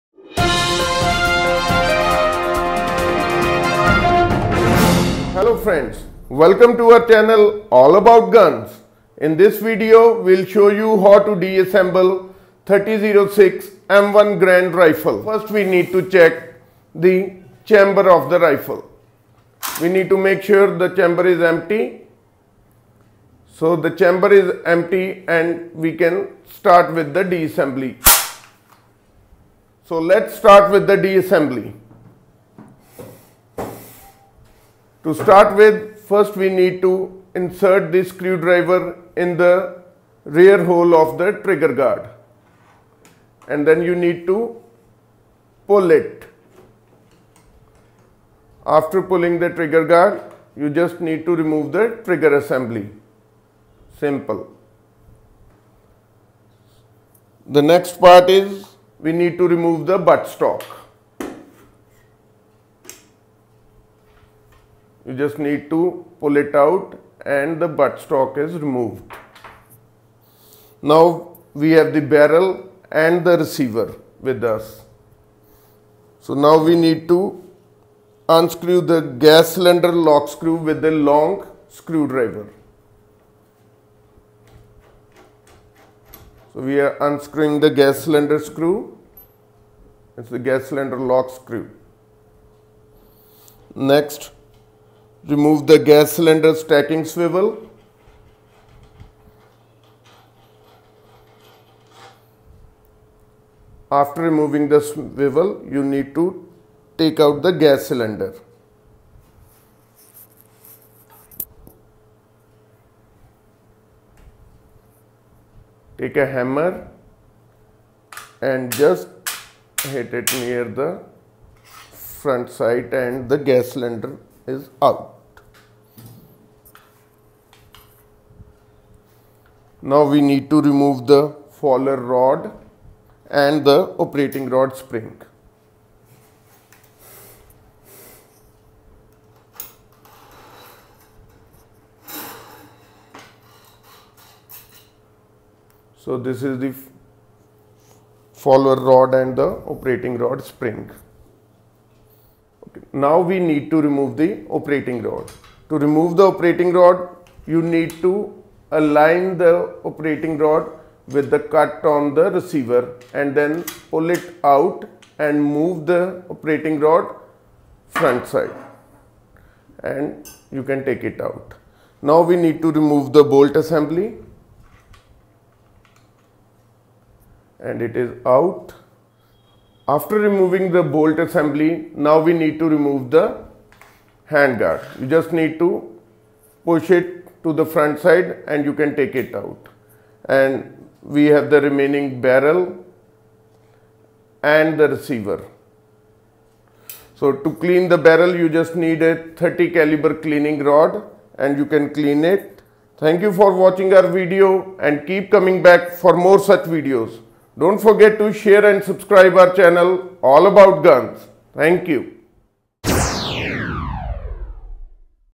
How To Disassemble 30-06 M1 Garand Rifle
Learn How To Disassemble 30-06 M1 Garand Rifle. DIY - Step by step Tutorial to help you clean and maintain your weapon.